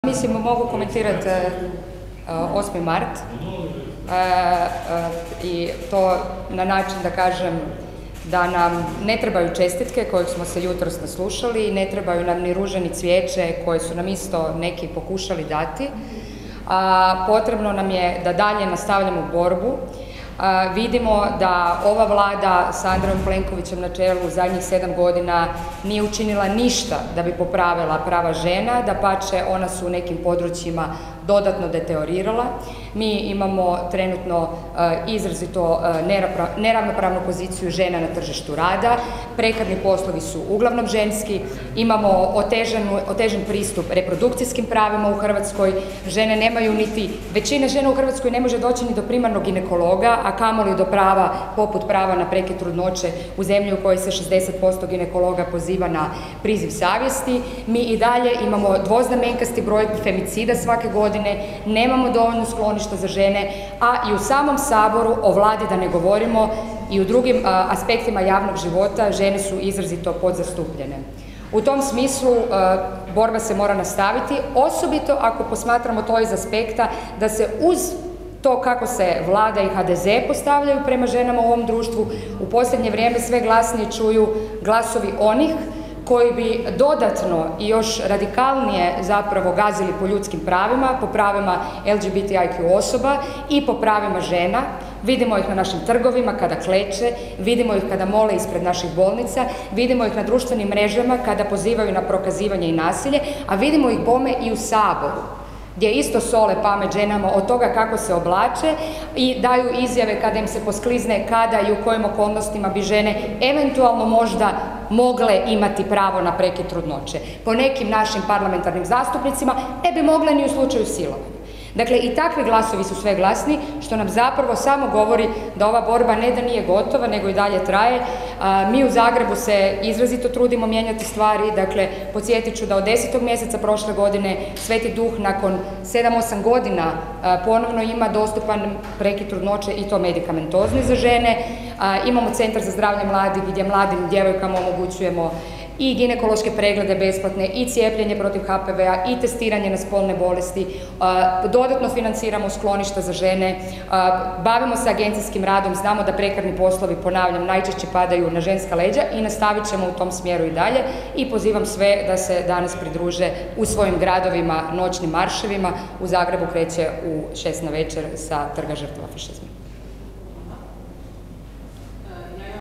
Ivana Kekin o Danu žena i pravima žena u Hrvatskoj
Na konferenciji za medije održanoj u Saboru zastupnica Ivana Kekin govorila je o Danu žena i pravima žena u Hrvatskoj.